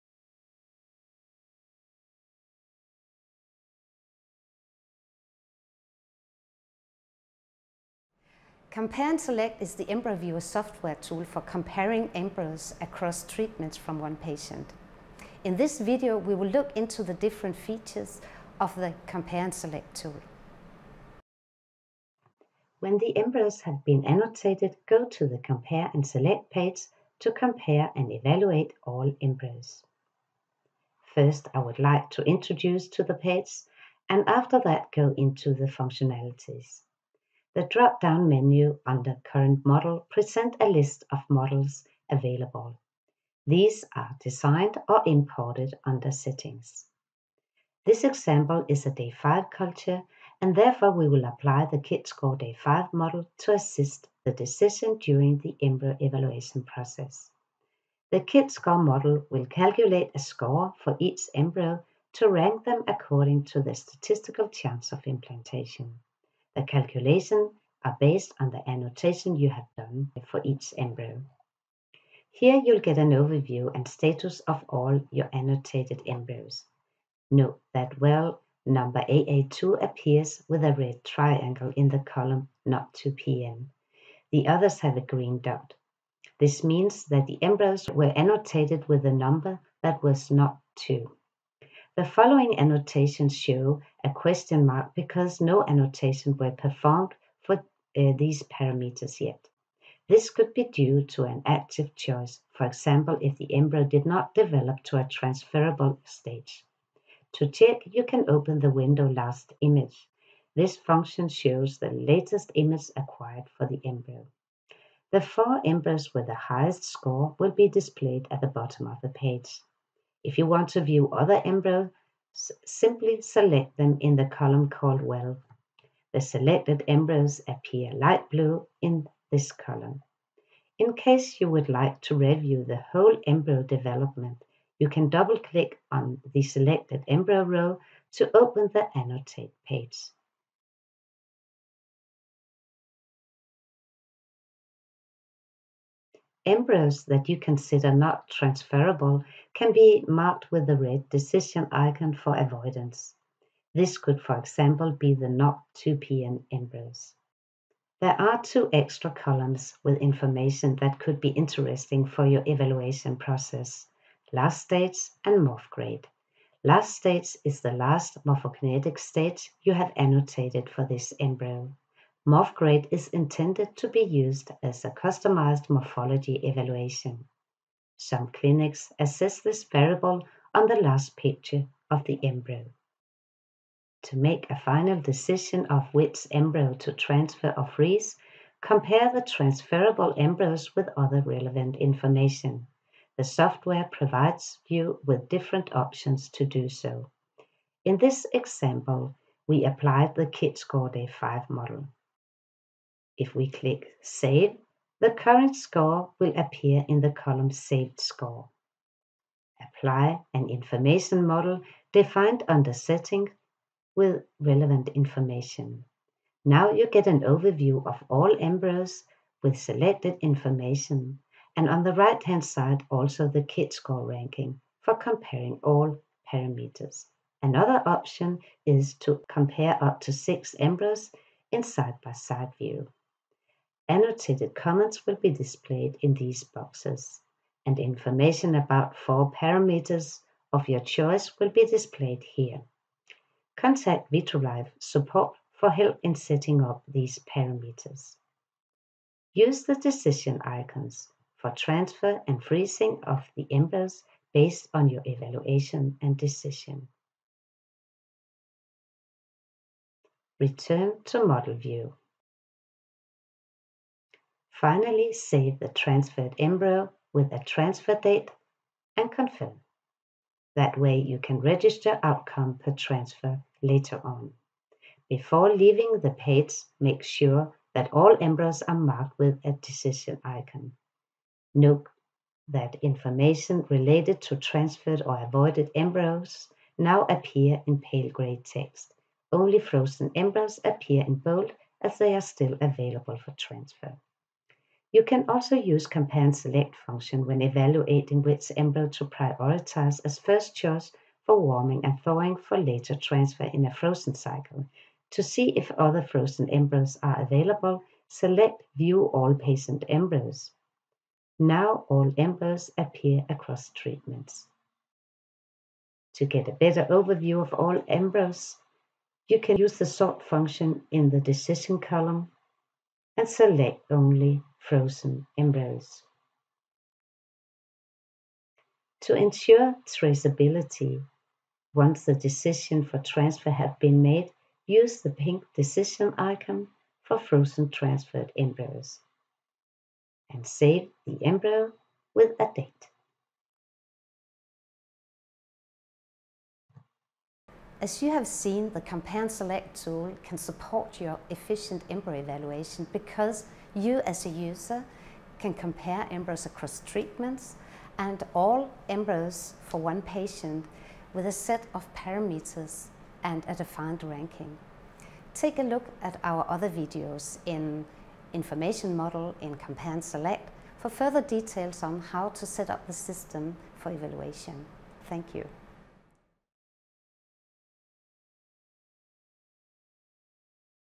Compare and Select – optimising embryo evaluation
“Compare & Select” is the EmbryoViewer software tool for comparing all embryos across a patient’s treatments. In this video, we will look into the different features of the “Compare & Select” tool.